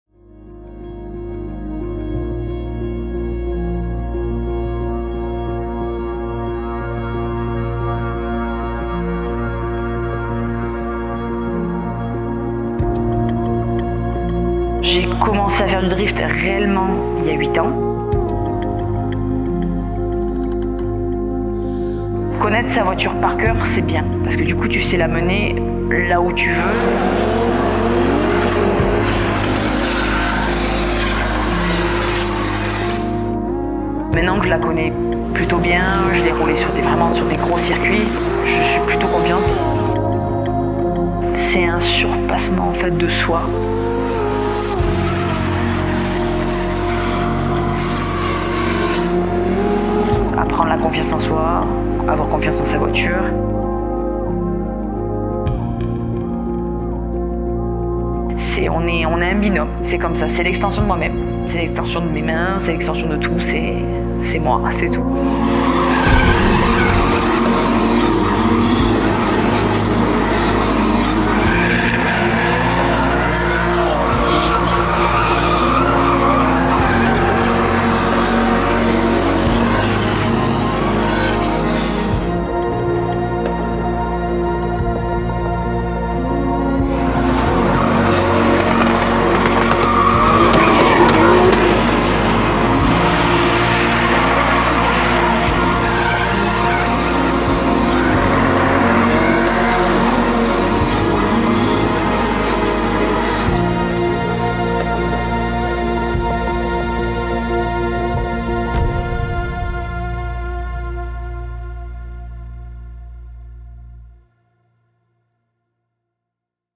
NANAKI - BMW E36 Drift Girl by Rec Slide
🚗💨 Nanaki, la pilote qui fait sensation depuis 8 ans ! un aperçu de son incroyable parcours, avec des moments forts sur les événements Slide.

| Panasonic S1H |
Directed by Rec Slide
Prod by Lynwood vision